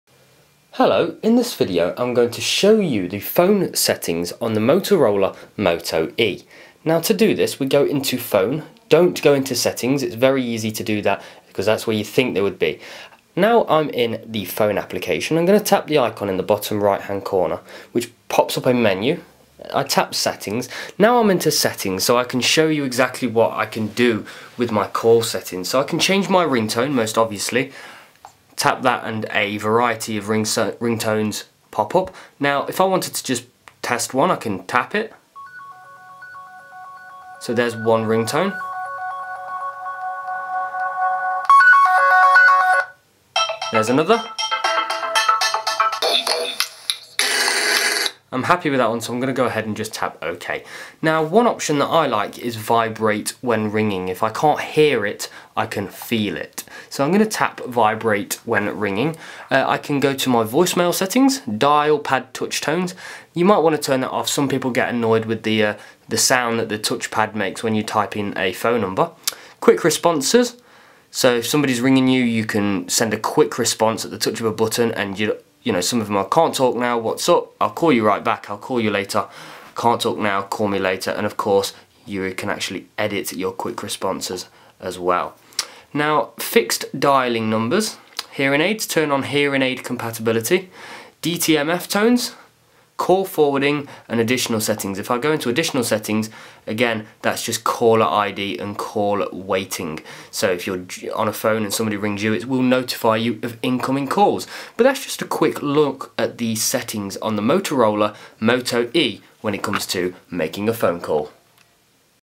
How To Manage Phone Settings - Motorola Moto E
A video how to, tutorial, guide on managing the phone settings on the Motorola Moto E.